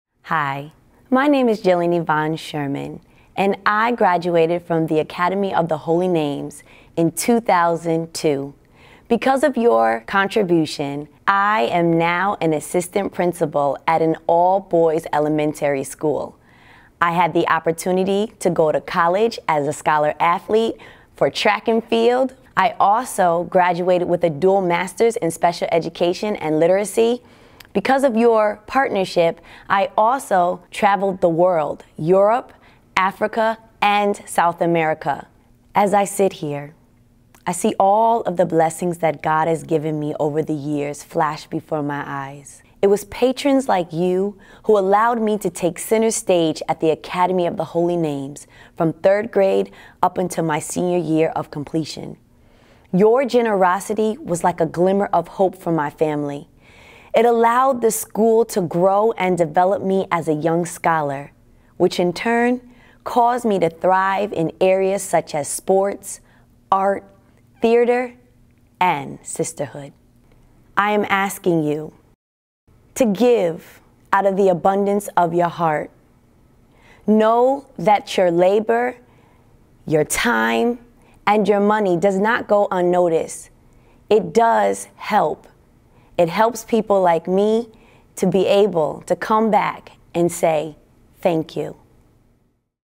Jillian Sherman, 2021
Jillian Sherman talks about her Holy Names experience.